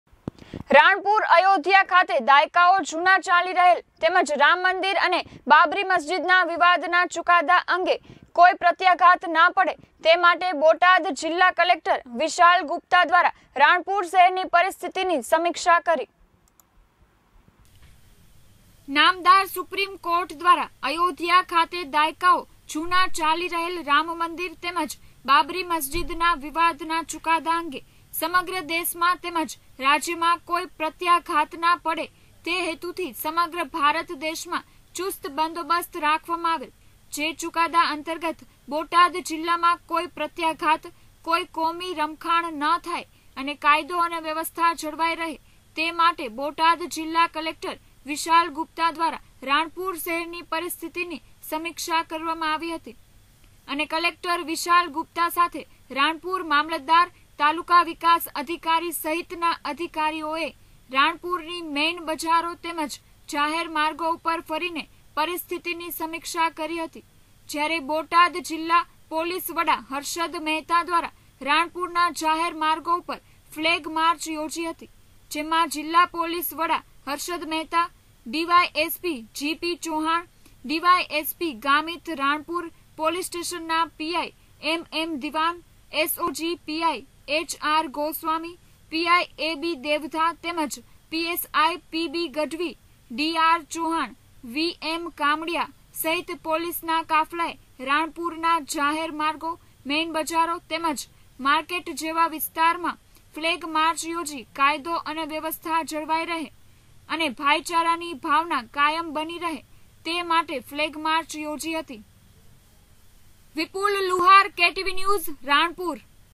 RANPUR: રાણપુર અયોધ્યા ખાતે દાયકાઓ જુના ચાલી રહેલ રામમંદીર તેમજ બાબરી મસ્જિદ...
RANPUR: રાણપુર અયોધ્યા ખાતે દાયકાઓ જુના ચાલી રહેલ રામમંદીર તેમજ બાબરી મસ્જિદના વિવાદના ચુકાદા અંગેના કોઈ પ્રત્યાઘાત ના પડે તે માટે બોટાદ જીલ્લા કલેક્ટર વિશાલ ગુપ્તા દ્રારા રાણપુર શહેર ની પરીસ્થીતીની સમીક્ષા કરી.